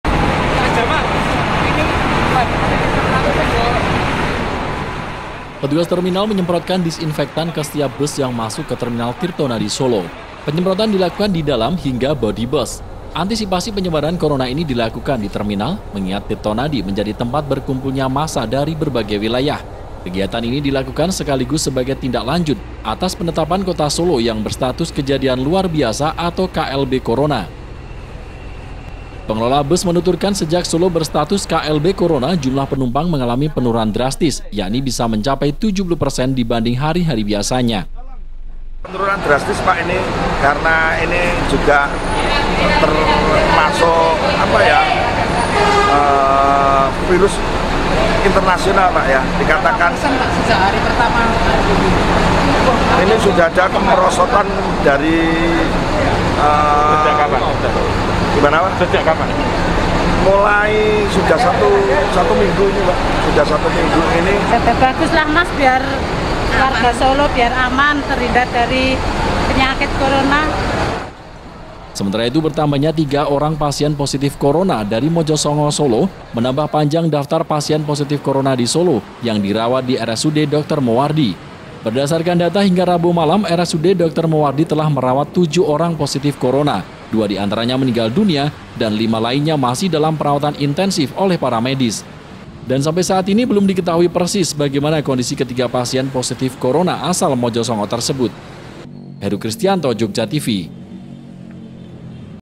BUS DI TERMINAL TIRTONADI SOLO DISEMPROT DISINFEKTAN- JOGJA TV
Berbagai upaya mencegah penyebaran virus corona terus dilakukan Pemerintah Kota Surakarta, salah satunya dengan melakukan penyemprotan disinfektan di Terminal Tirtonadi. Petugas menyemprotkan disinfektan ke setiap bus yang masuk ke terminal.